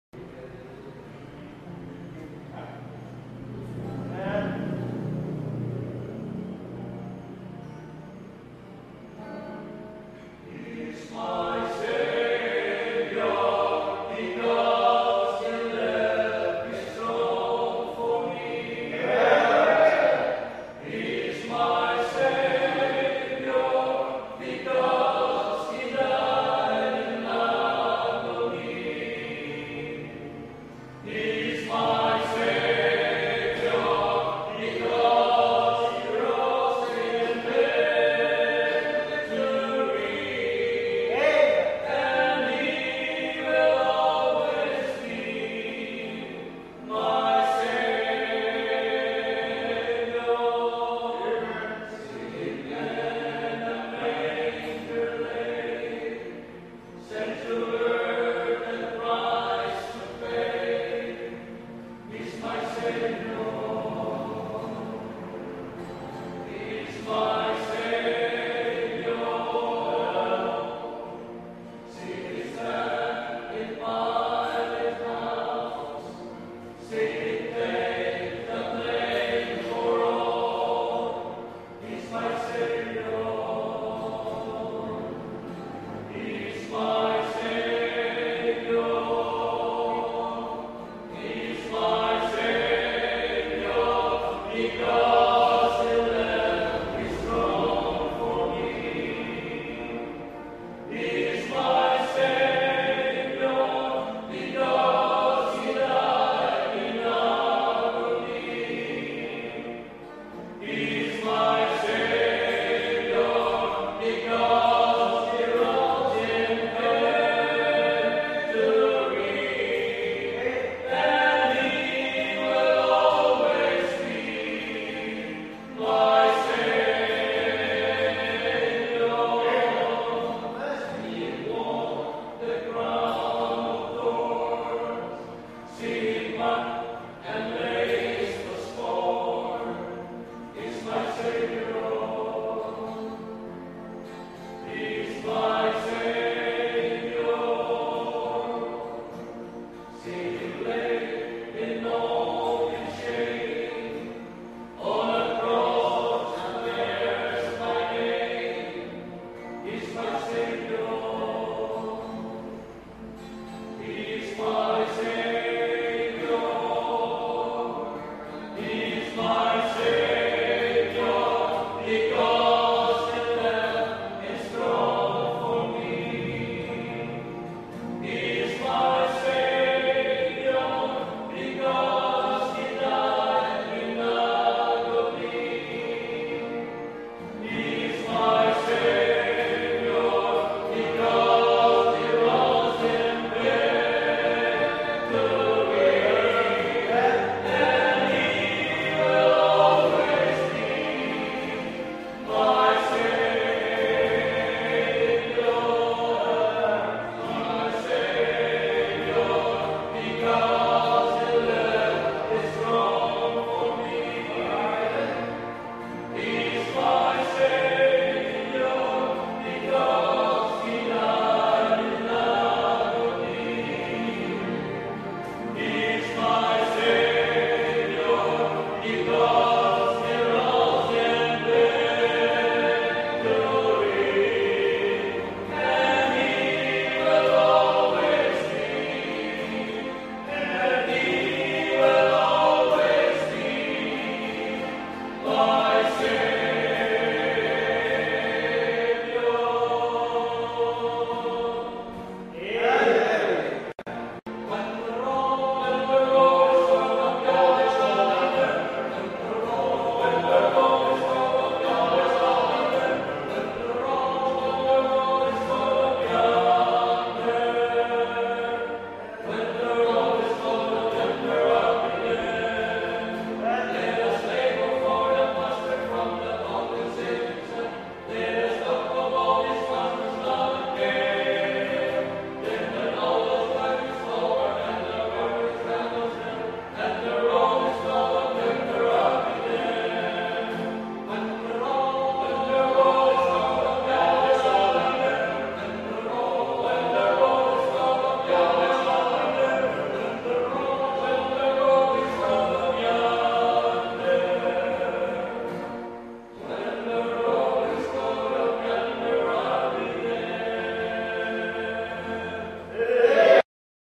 The VisMinLu Quartet sings at the 2nd Pastor's Conference at Fbbc Mission Center in Bohol
The VisMinLu Quartet is teamed up by Pastor Chiong from Oroquieta City Mindanao, Pastor Hoyohoy from Tubigon Bohol, Pastor Blanco from Panglao Bohol and Pastor Lanciola from Lapu-lapu City Cebu. VisMinLu stands for Visayas Mindanao Luzon and Bible Baptist Churches in these area registered a Fellowship to SEC under the name of VisMinLu